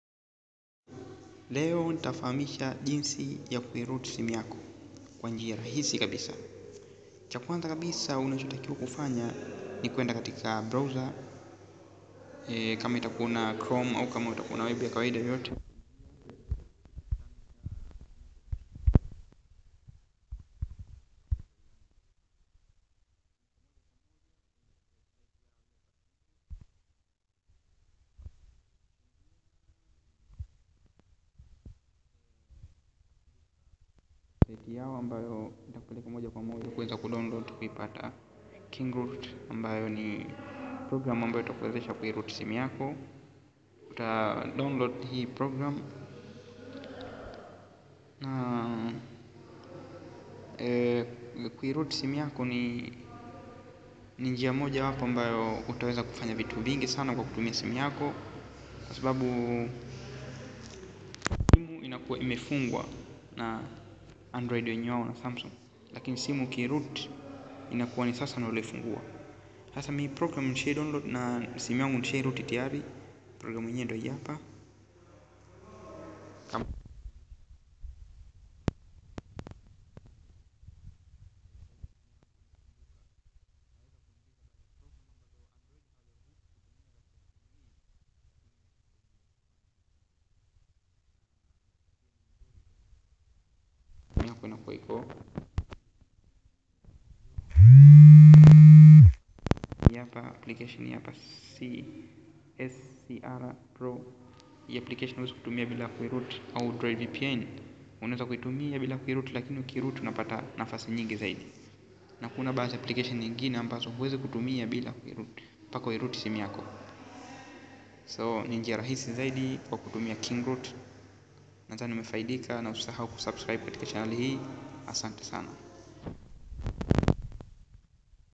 Jinsi ya kuroot simu yako kwa njia nyepesi kabisa
Jinsi ya kuiroot simu yako kwa njia nyepesi kabisaa tofauti na kutumia kompyuta njia hii unaweza kuroot kwa kutumia simu tu na utaweza kufanya hivyo kwa kutumia app ya kingroot